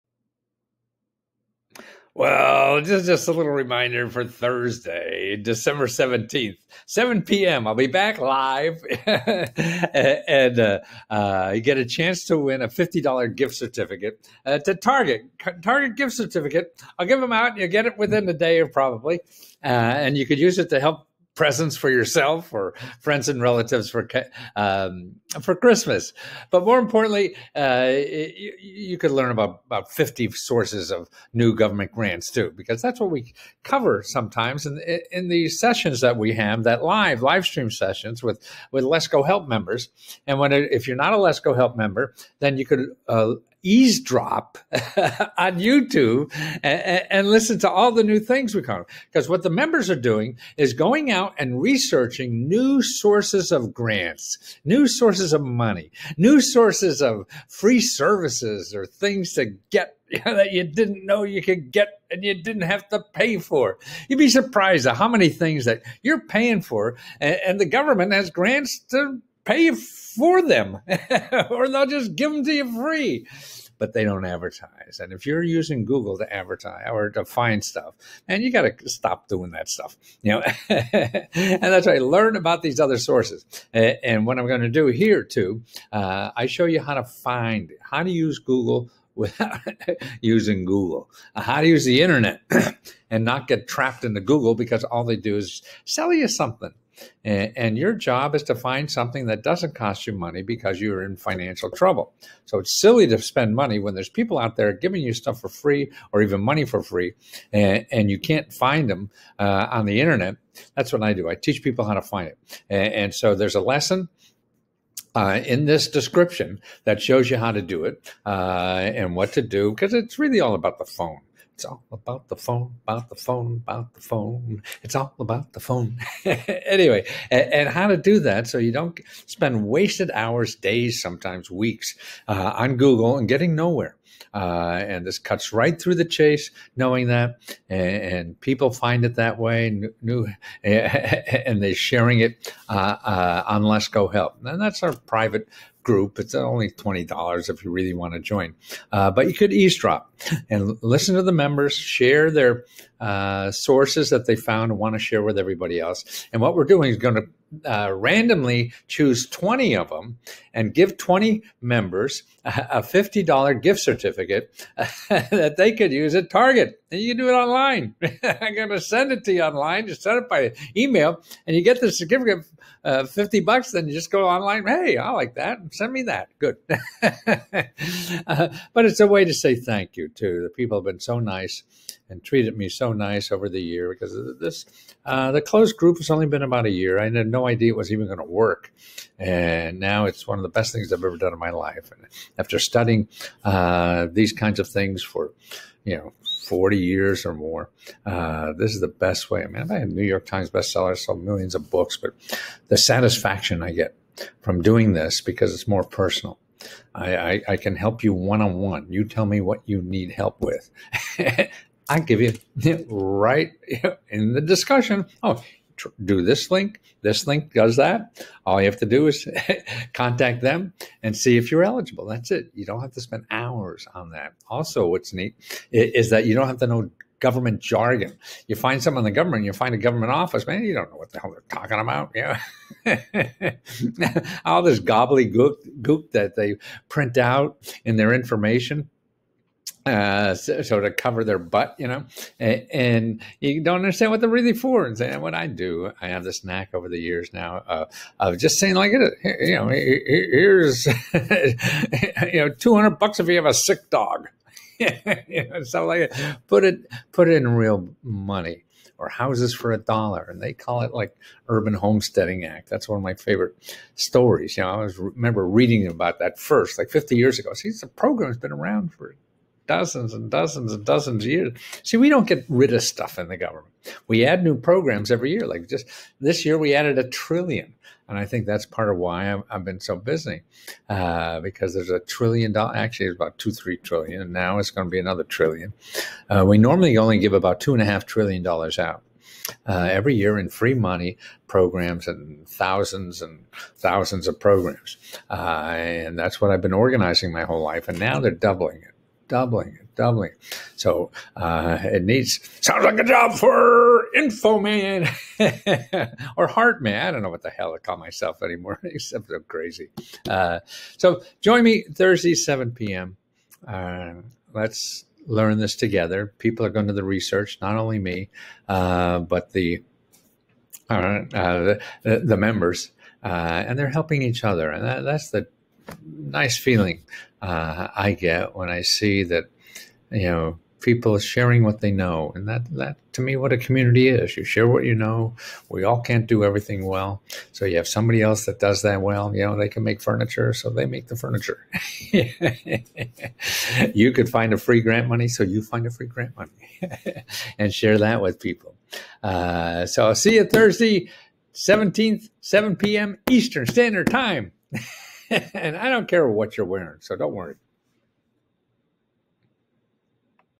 Thursday Night Learn 30 New Covid Personal Grant Sources and Win 20 $50 Target Christmas Gift Cards
Review These Research Tips To Make Sure You Are Getting the Most Out to Gov’t Money Sources

#1...Don’t Start With Google ...Start With Local Organizations Who Get Grants To Help Solve Your Financial Problems

#3..Use Internet Robots To Search The Internet For NEW  Covd Personal Grants While You Sleep

500+ Sources of Covid Emergency Personal Grants Individuals, Artists, Business and Non-Profits